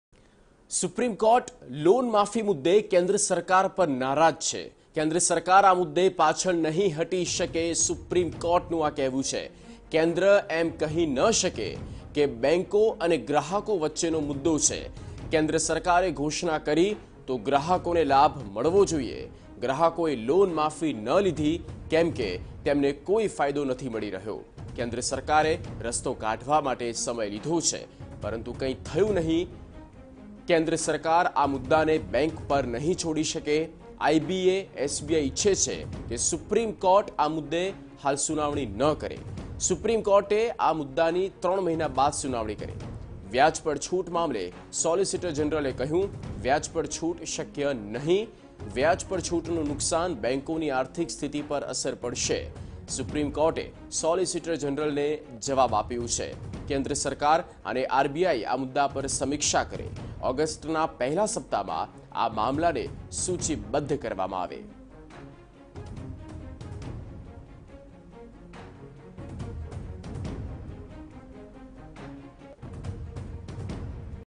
Loan moratorium case: SC defers consideration of waiver of interest on EMI till Aug 1st week| TV9
Loan moratorium case: SC defers consideration of waiver of interest on EMI till Aug 1st week | TV9News